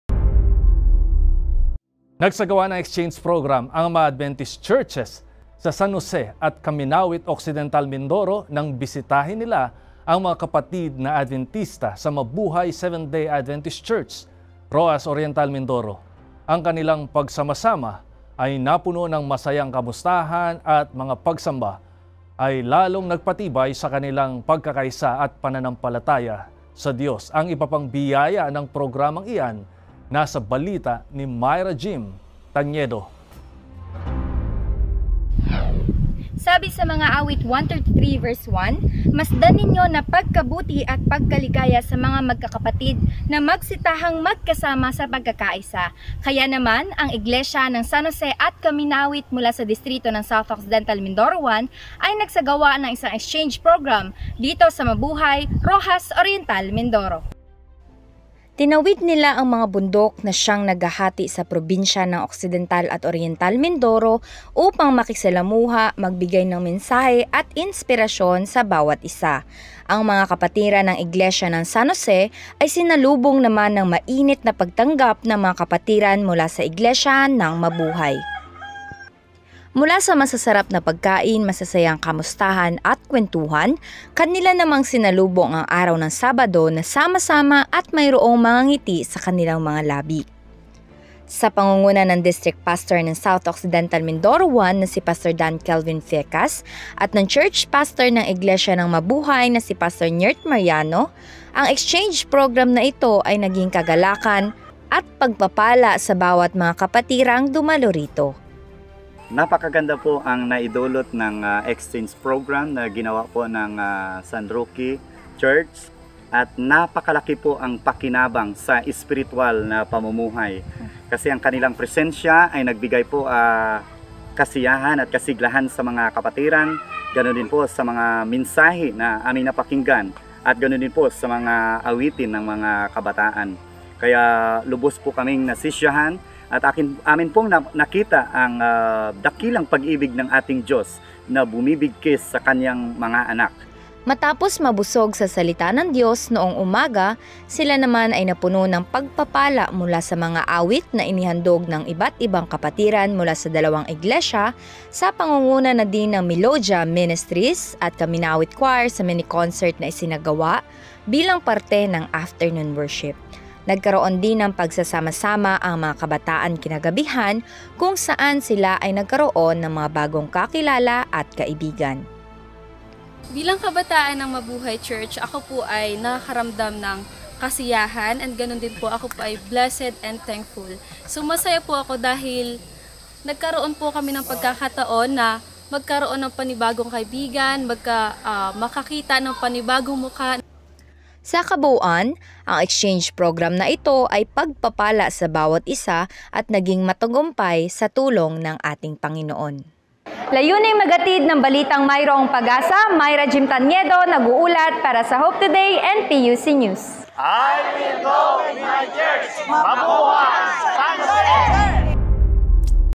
San Jose at Caminawit Churches nagdaos ng Exchange program sa Mabuhay SDA Church
Ang kanilang pagsasama-sama ay napuno ng masasayang kamustahan at sa pagsamba ay lalong nagpatibay sa kanilang pagkakaisa at pananampalataya sa Diyos.
Ang iba pang biyaya ng programang yan, nasa balita ni Myra Jeam Tañedo…